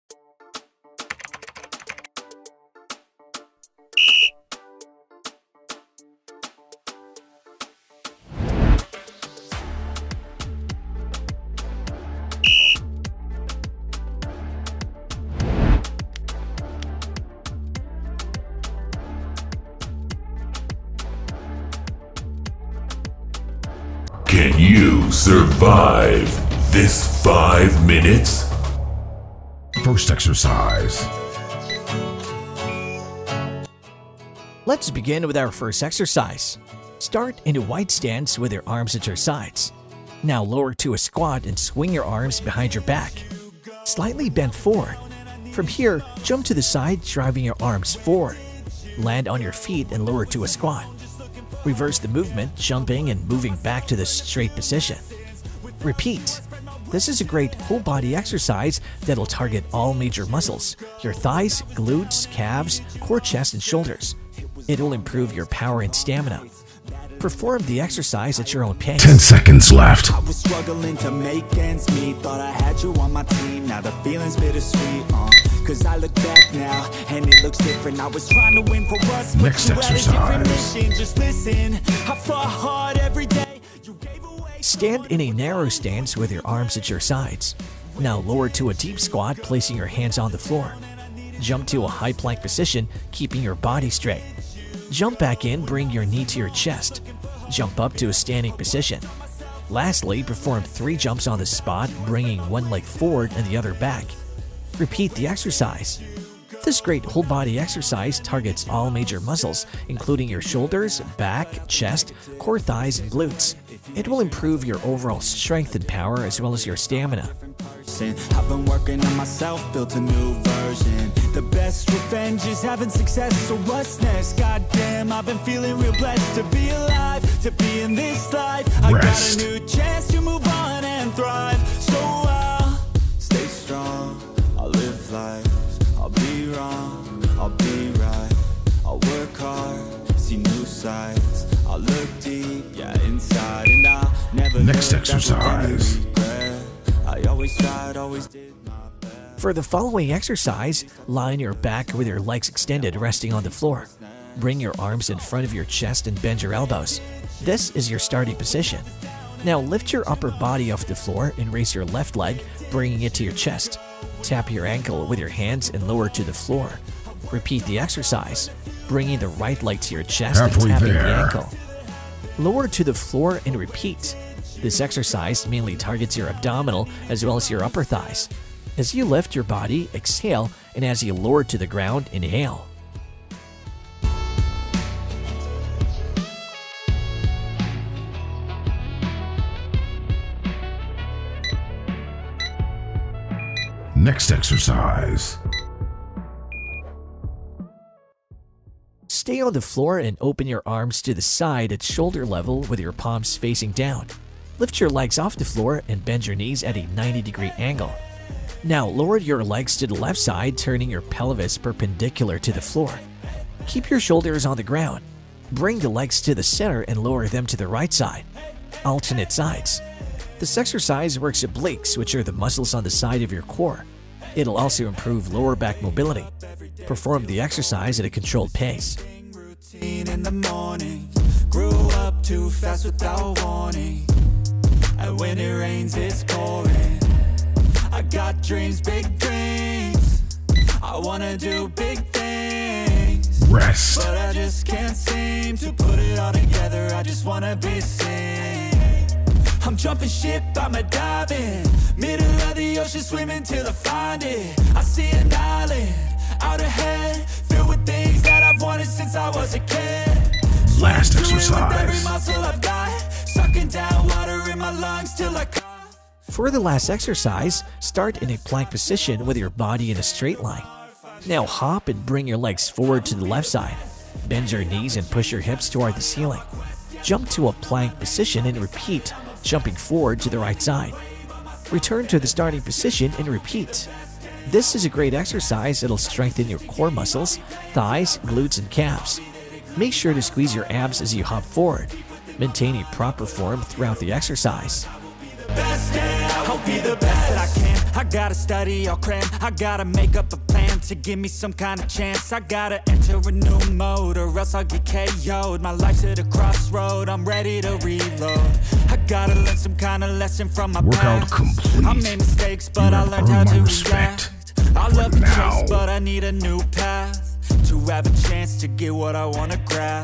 See What Happens If You Do This For 5 Min Every Morning
0:30 Side Hop Skis
1:19 Shuffle Burpees
2:24 Half Jackknives
3:14 Bent Leg Wipers
4:20 Twisting Pistons